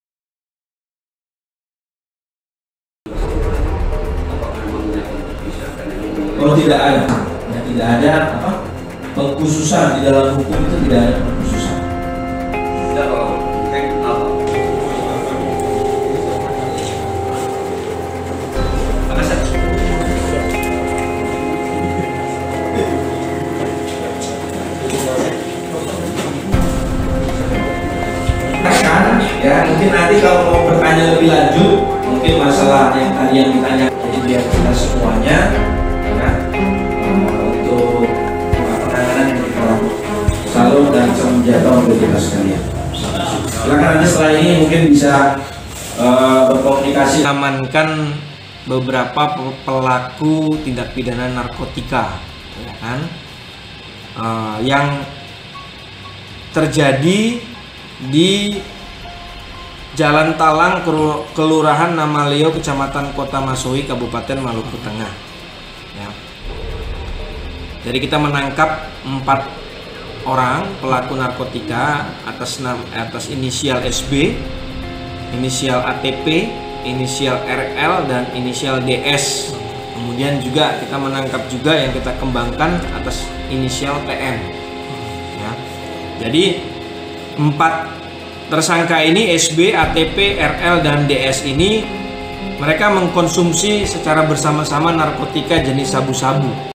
POLRES MALUKU GEREBEK OKNUM ANGGOTA DPRD BERSAMA SEJUMLAH WARGA SAAT SEDANG PESTA NARKOBA
Satuan Narkoba Polres Maluku Tengah menggerebek pesta sabu-sabu yang dilakukan oknum Anggota DPRD Maluku Tengah bersama 3 orang warga Kota Masohi, Kabupaten Maluku Tengah. Polisi juga menangkap seorang tersangka pengedar narkoba tersebut.

Dari tersangka polisi mengamankan sejumlah barang bukti narkoba diantaranya, satu paket sabu-sabu, alat hisap sabu-sabu atau bong, satu buah pipet kaca, serta barang bukti lainnya.